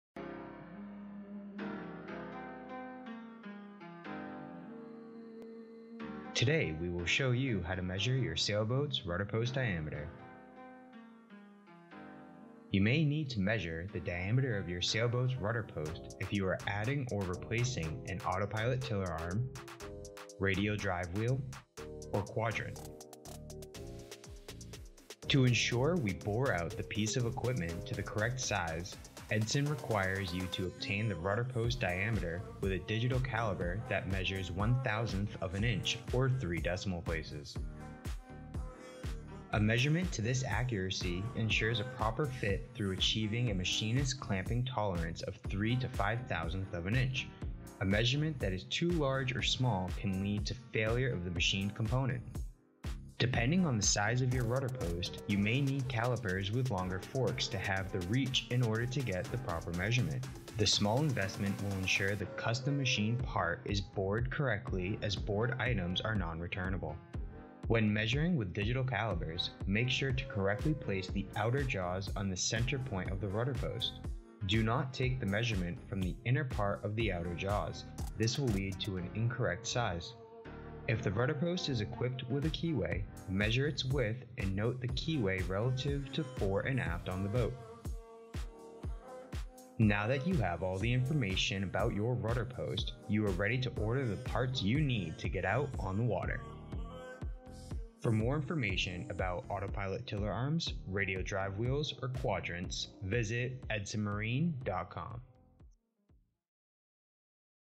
Measuring Your Sailboat Rudder Post Diameter
In this video, we will go over how to properly use a digital caliper to measure the diameter of a sailboats rudder post.  This video is for sailors that are planning on replacing or adding a new Edson Autopilot Tiller Arm, Radial Drive Wheel, or Quadrant.